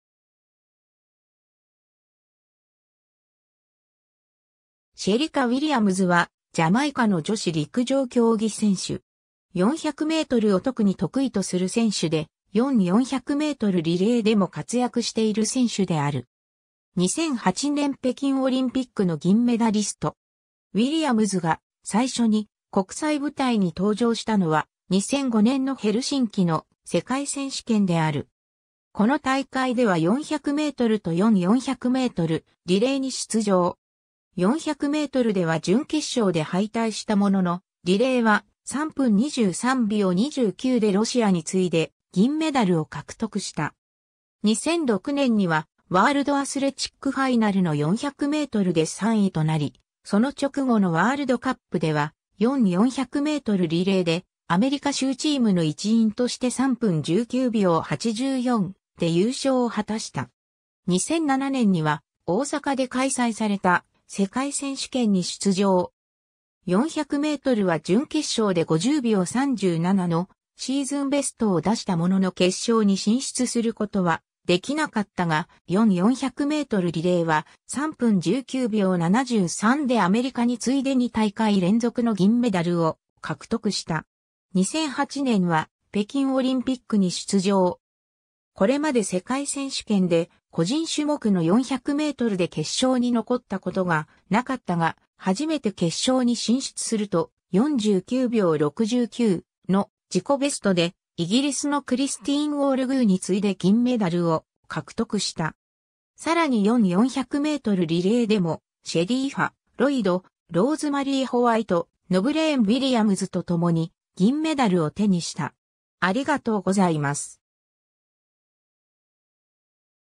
シェリカ・ウィリアムズ
セント・エリザベス教区出身の人物
シェリカ・ウィリアムズ（Shericka Williams、1985年9月17日 - ）は、ジャマイカの女子陸上競技選手。
400mを特に得意とする選手で、4×400mリレーでも活躍している選手である。
2008年北京オリンピックの銀メダリスト。
 ウィリアムズが、最初に国際舞台に登場したのは、2005年のヘルシンキの世界選手権である。
この大会では400mと4×400mリレーに出場。
400mでは準決勝で敗退したものの、リレーは、3分23秒29でロシアに次いで銀メダルを獲得した。
2006年には、ワールドアスレチックファイナルの400mで3位となり、その直後のワールドカップでは、4×400mリレーでアメリカ州チームの一員として3分19秒84で優勝を果たした。
 2007年には、大阪で開催された世界選手権に出場。
400mは準決勝で50秒37のシーズンベストを出したものの決勝に進出することはできなかったが、4×400mリレーは、3分19秒73でアメリカに次いで2大会連続の銀メダルを獲得した。
 2008年は北京オリンピックに出場。
これまで世界選手権で、個人種目の400mで決勝に残ったことがなかったが、初めて決勝に進出すると、49秒69の自己ベストで、イギリスのクリスティーン・オールグーに次いで銀メダルを獲得した。
さらに4×400mリレーでも、シェリーファ・ロイド、ローズマリー・ホワイト、ノブレーン・ウィリアムズとともに銀メダルを手にした。